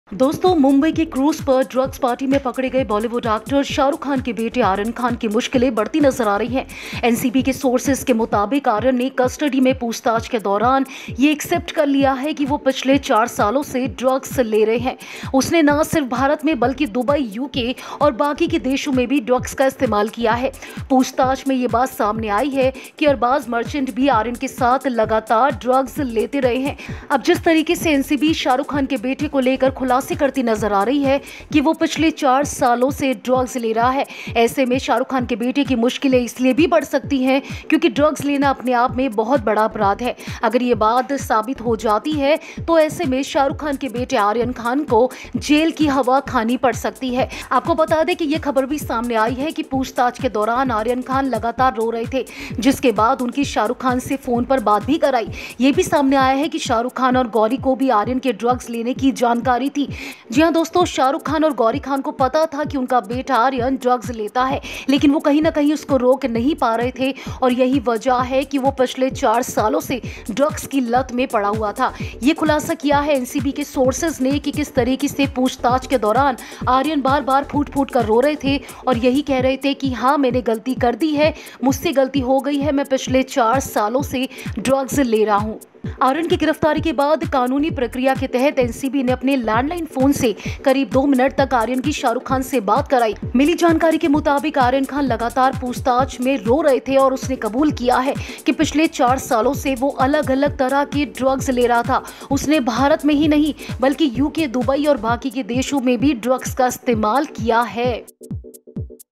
Aryan Drugs Case: बीते 4 सालों से Drugs ले रहा है आर्यन , शाहरुख ने फोन पर की बात | FilmiBeat
Aryan Khan revealed during the interrogation that he had been consuming drugs for four years.Watch Out